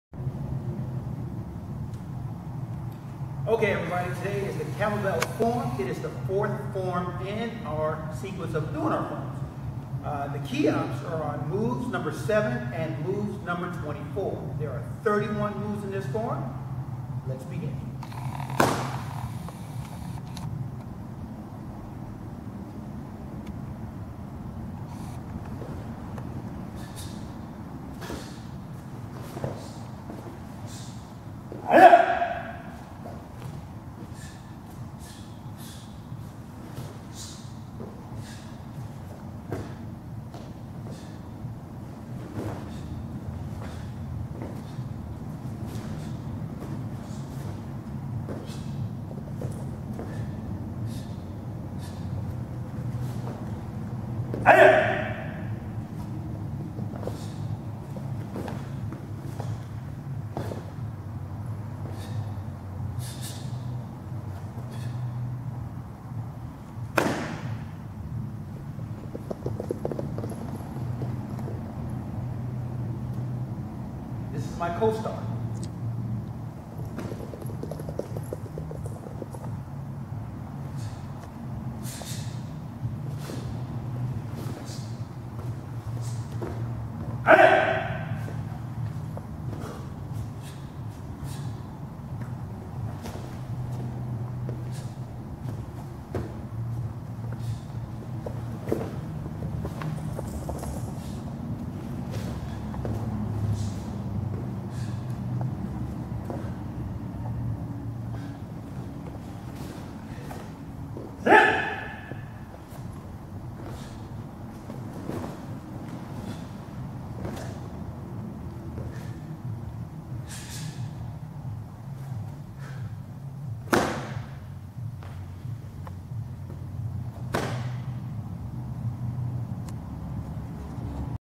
Camo Belt Form
Camo Belt Form - 31 moves, kihaps on 7 and 24